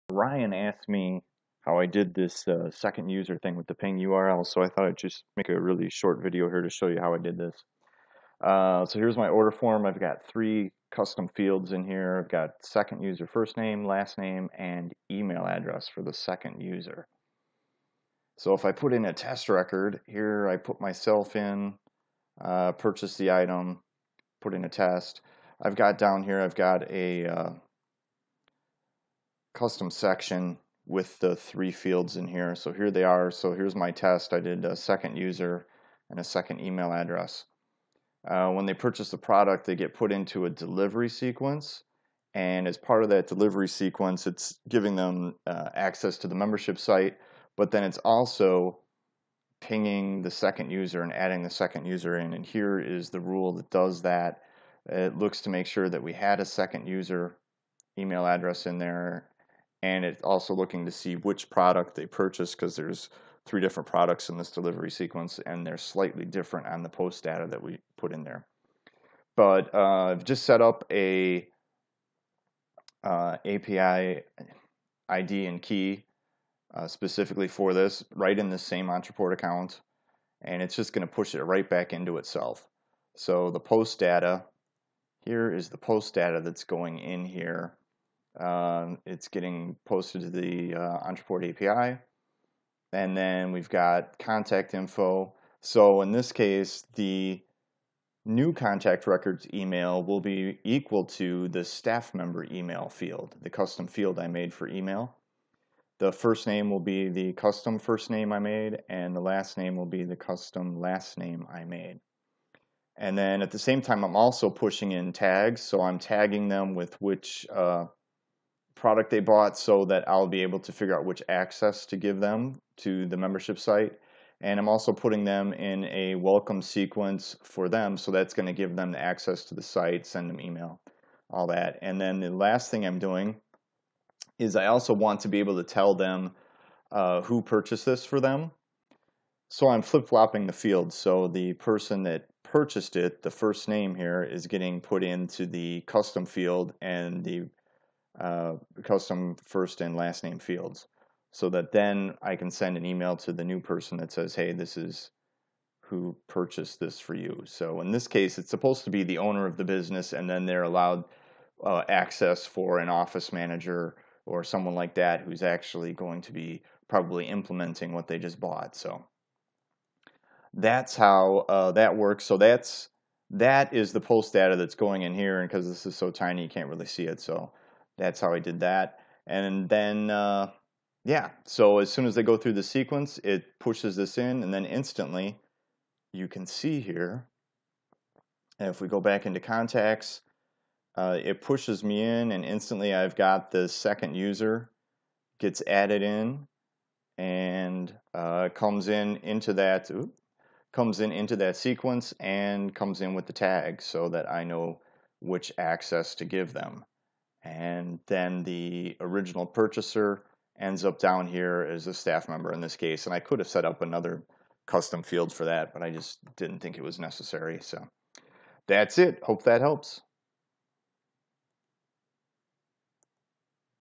Ontraport Training – How To Add Two Users With One Order Form
Did you know you can add two users to your Ontraport system using one order form?

It’s a little-known trick that I came up with when a client needed to add a second user for a paid membership site.